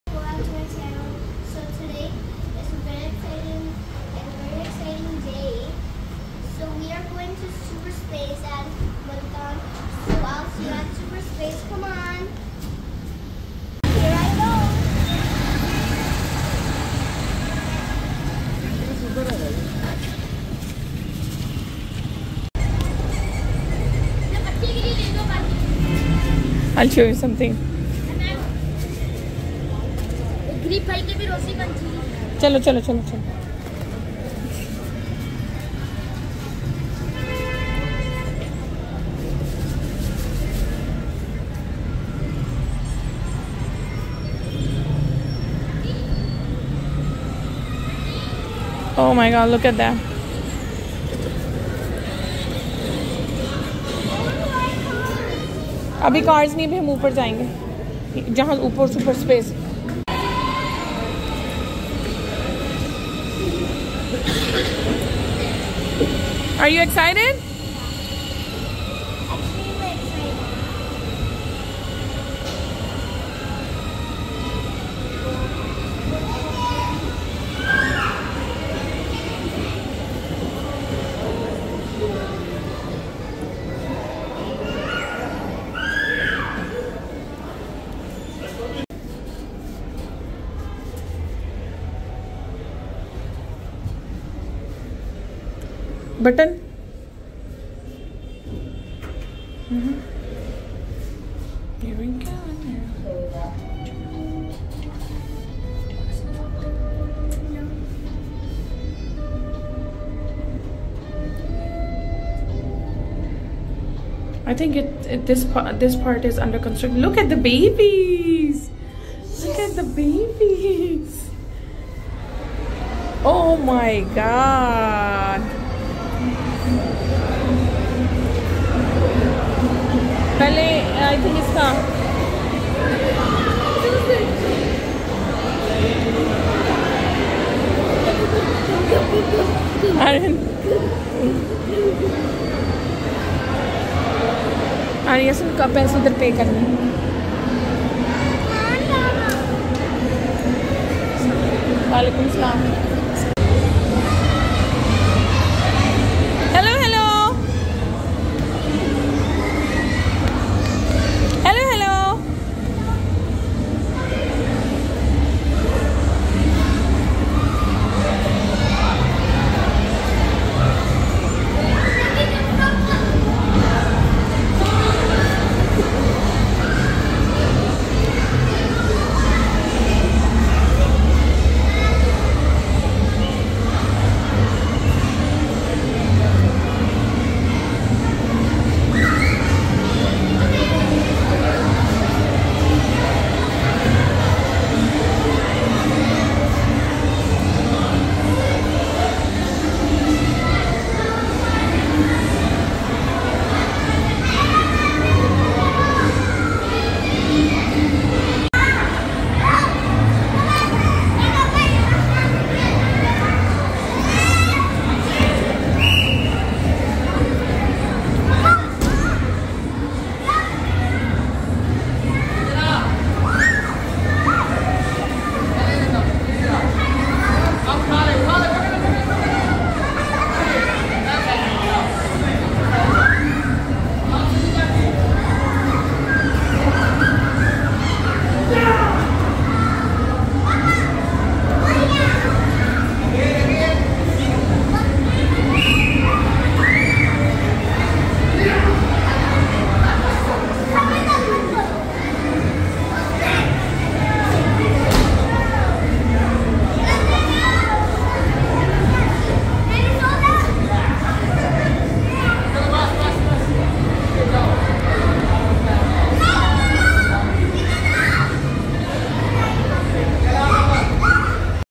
Super Space Multan | Pakistan Visit
In this video we will enjoying one of the best Pakistan indoor theme park.